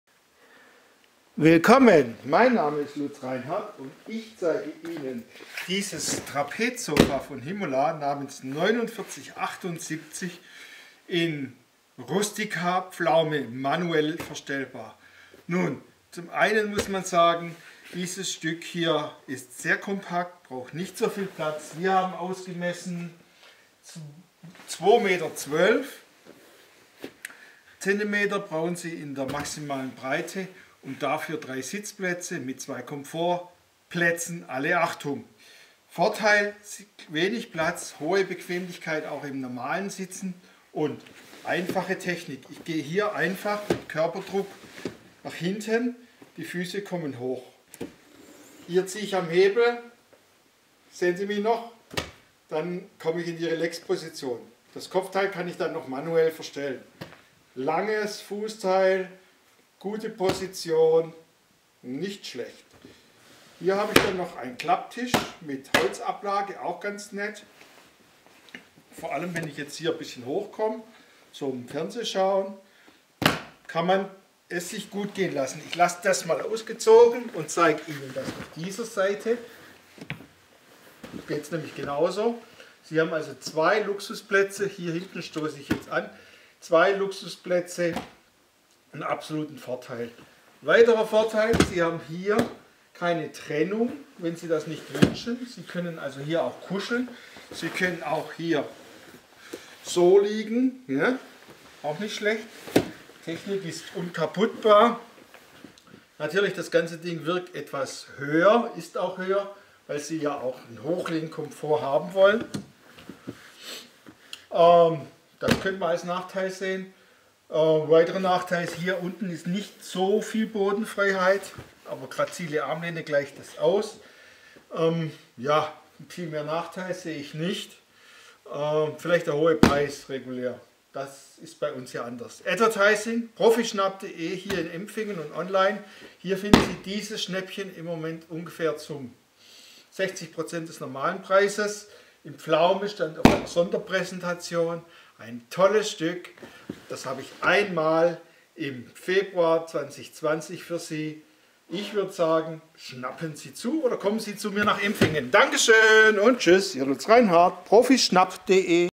Himolla 4978 Cumully Trapezsofa 3 tlg t manuell Vollfunktion Leder Longlife Rustika Pflaume
Auf einer Sonderpräsentation wurde ...
... diese Trapezecke gezeigt: eine einmalige Ausstellung, vielleicht bei einer Bank? Dann kam die Kombi ins 1b-Warenlager, von dort zu uns. Die Wohnkombination besitzt eine manuelle Verstellung des Rückens per Gasdruck: Anhalten in jeder Position möglich. Die Fußstütze fährt sehr einfach durch Körperdruck aus, für ein Höchstmaß an Bequemlichkeit. Zusätzlich haben beide Sitze eine manuelle Verstellung für den Kopfbereich, für echten Hochlehnkomfort. Die Garnitur ist ergonmisch fester gepolstert. Der ergonomische Sitzkomfort wird Sie begeistern, die Leichtgängigkeit der Funktion auch. Der klappbare Mitteltisch ist praktisch und hat doppelten Nutzen für Sie: Zum Querliegen hochgeklappt und als Ablage, wenn der Rücken runtergeklappt ist. Das Leder ist pflegeleicht und hautsymphatisch. Mit Keralux-Lederpflegeset für Longlifeleder leicht in Schuß zu halten.

Sie suchen ein bequemes Spitzensofa? Dann haben Sie sie jetzt gefunden!